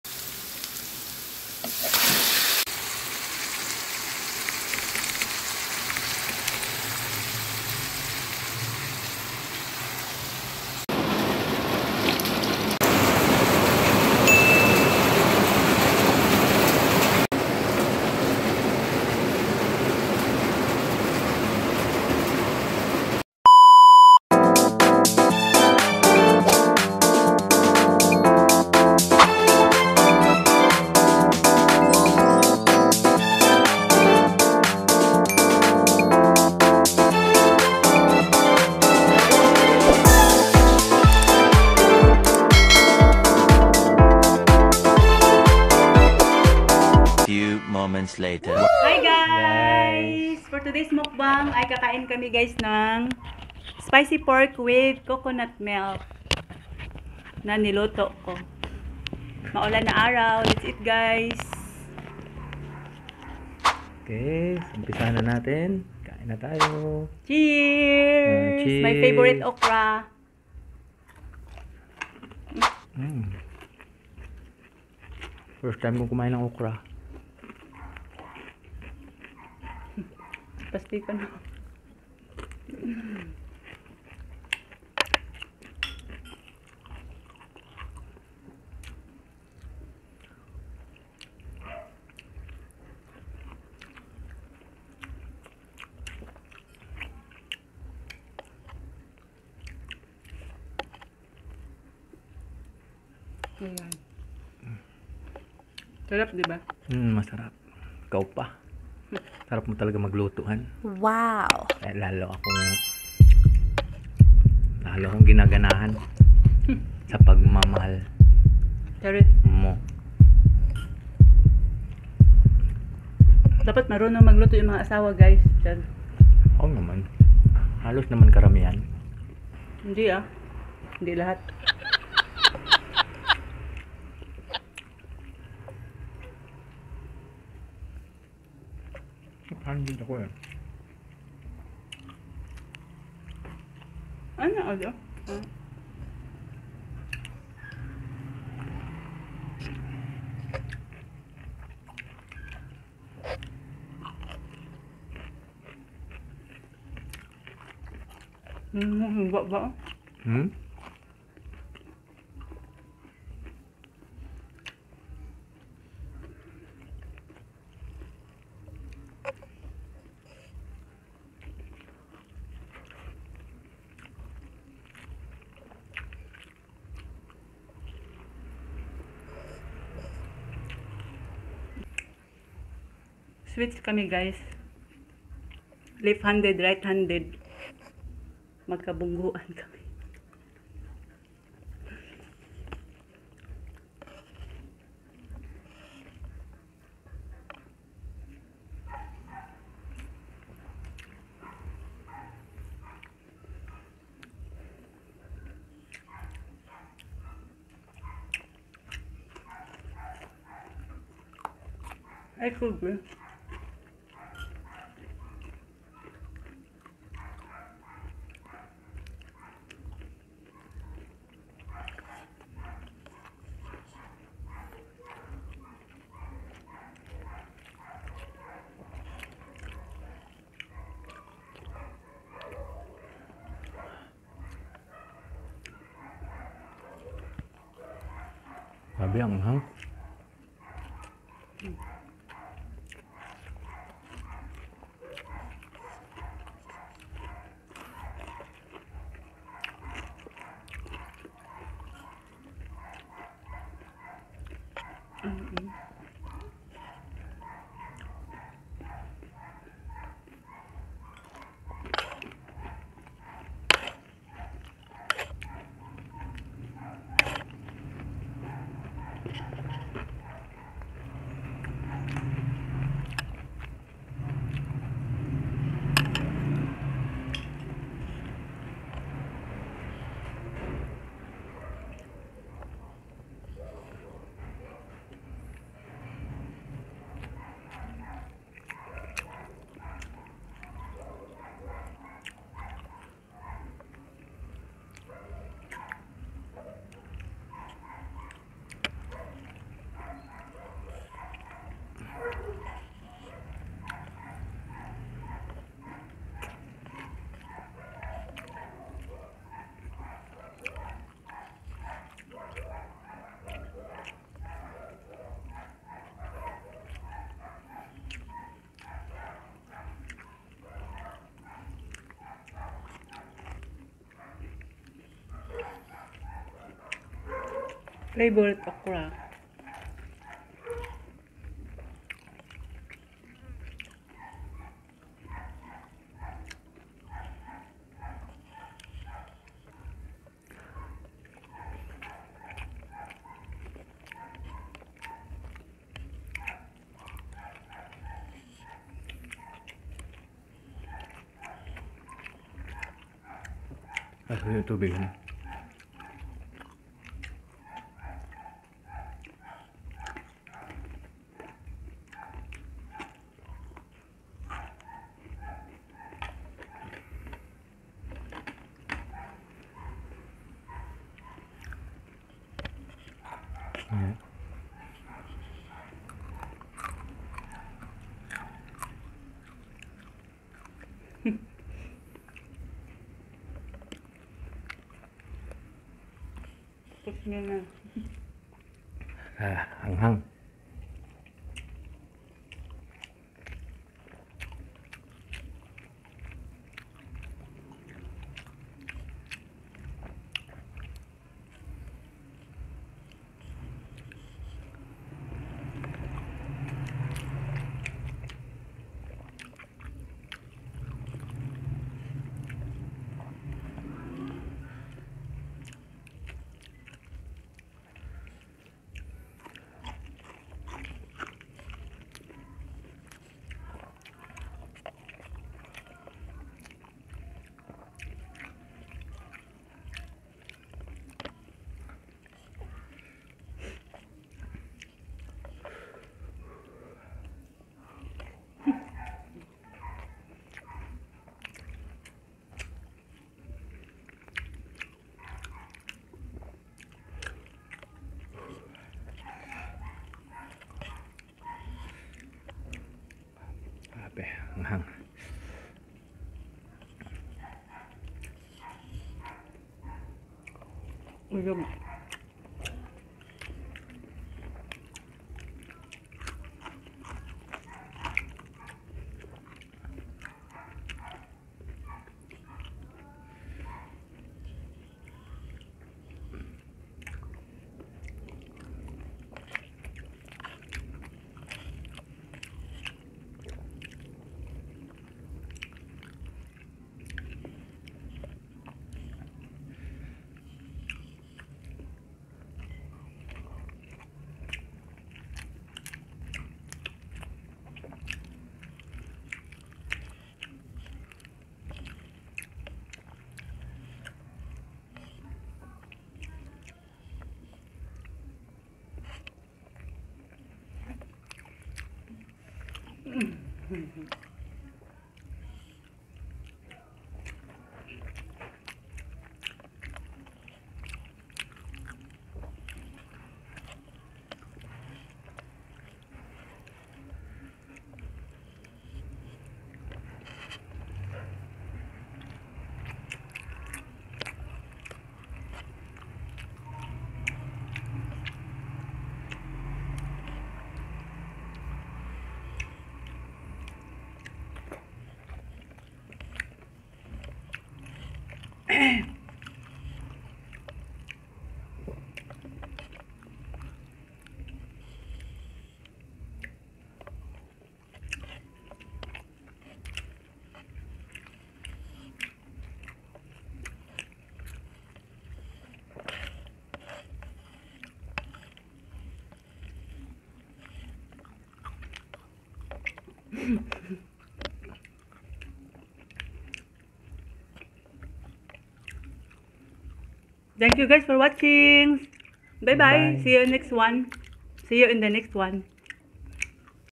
My spicy pork with coconut milk | Pinoy Mukbang | Eating Sounds
My version of cooking spicy pork with coconut milk..